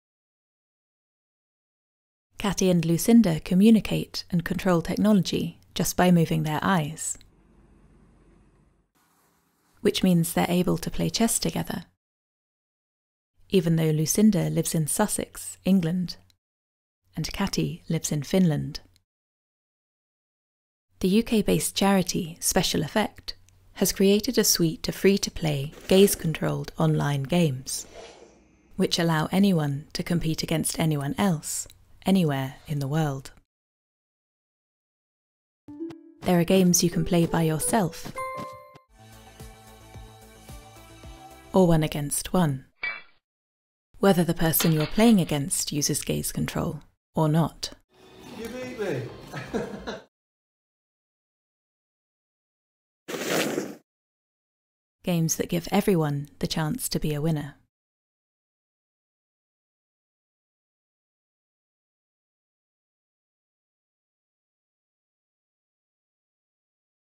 Kati and Lucinda play chess - just by moving their eyes
Checkmate: here’s Kati in Finland and Lucinda in Surrey playing chess together - just by moving their eyes. They’re playing courtesy of SpecialEffect's Eye Gaze Games website, an accessible suite of eight online multiplayer eye-controlled web games that make global play possible between gamers with a huge range of physical abilities, giving everyone the chance to be a winner.

Kati and Lucinda, who are both accomplished eye gaze users, provided invaluable advice during development of the platform. Although the games on the site are optimised for eye control, the accessibility settings purposely accommodate a wide range of other inputs including switches, touchscreen and a mouse control pointer, enabling inclusive and competitive play between almost anyone.

The launch of the Eye Gaze Games website extends the charity’s mission to help people with severe physical challenges all around the world to play video games to the best of their abilities. The games open up a new era of gaming inclusion for disabled players and represent a model for developers which highlights the additional enjoyment that can be gained from including gaze-control in their games.

All the games on the website are freely playable. Along with Chess, the others are Mole Miners, Four-in-a-Row, EyeDrive, Draughts (Checkers), Solitaire, Sudoku and Sudoku Monsters. They can be played using any Windows eye gaze system which offers mouse control. More games will be added to the website as they are developed.

SpecialEffect are a UK-based charity that help people with physical disabilities to play video games.

Video by Cara Jessop